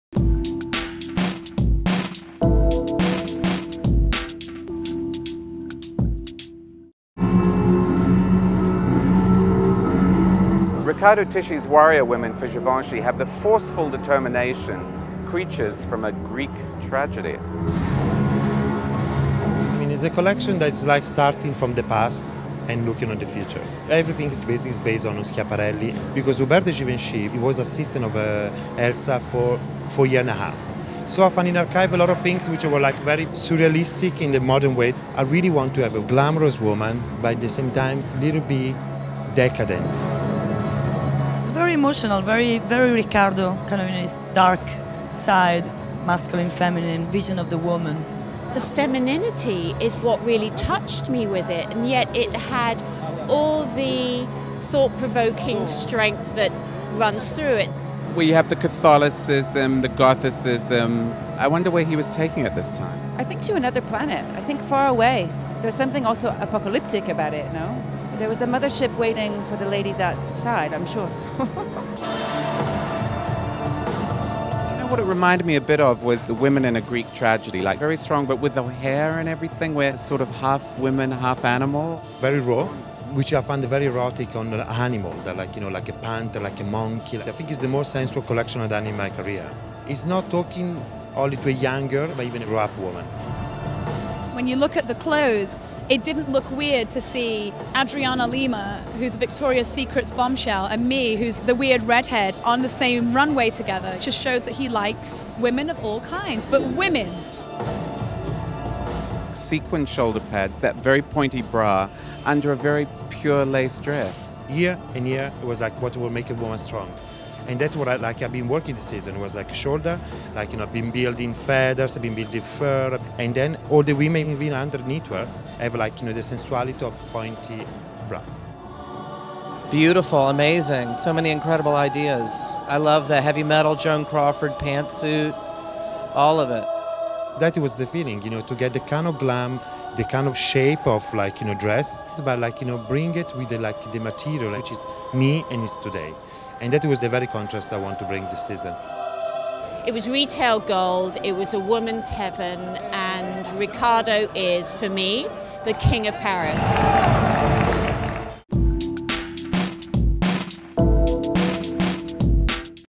Fashion Show - Givenchy: Fall 2009 Ready-to-Wear
Runway, backstage, and front-row footage from the Paris show.  Visit Style.com for more runway shows, fashion trends, shopping guides, and news about models and designers.

Starring: Riccardo Tisci